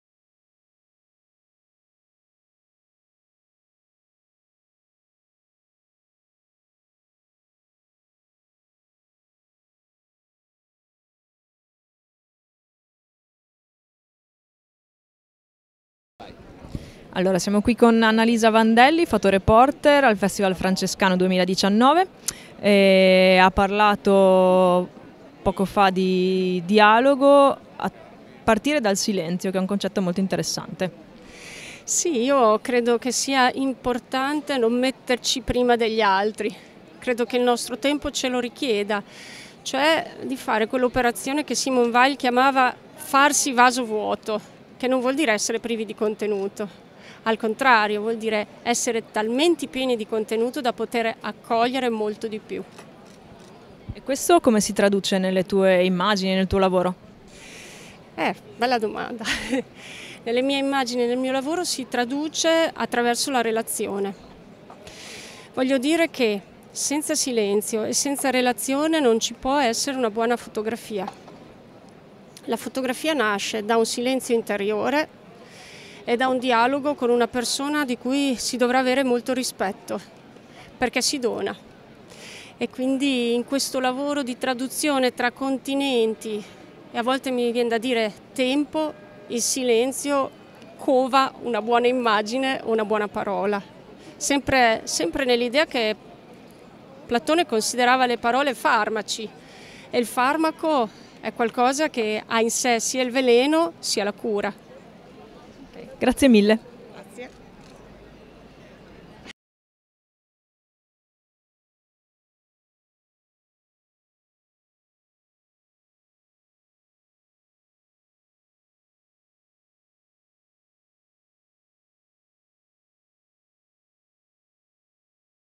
Intervista ad Annalisa Vandelli, Fotoreporter
I suoi articoli e le sue fotografie sono stati pubblicati su diverse testate come Il Corriere della Sera, Rai, Repubblica.tv, Avvenire, L’Espresso, Huffington Post. Dal 2015 insegna fotogiornalismo presso l’Università di Roma 3. La sezione pontificia Migranti e Rifugiati utilizza le sue immagini abbinandole alle parole di papa Francesco in tema di promozione integrale della persona, soprattutto migrante. Ha indagato situazioni di emergenza e paesi cosiddetti “in via di sviluppo” come Etiopia, Nicaragua, Tunisia, Guatemala, Pakistan, Territori Palestinesi, Kenya, campi profughi saharawi, Egitto, Albania, El Salvador, Giordania, Libano. Molte le mostre dedicate ai suoi racconti visivi; In un vortice di polvere, curata da Tatiana Agliani e dal maestro di fotogiornalismo Uliano Lucas, è stata recentemente esposta a Palazzo d’Accursio dal Green Social Festival di Bologna. Il suo ultimo volume fotografico s’intitola Per puro splendore. In viaggio con Nexus (Editrice Socialmente 2018).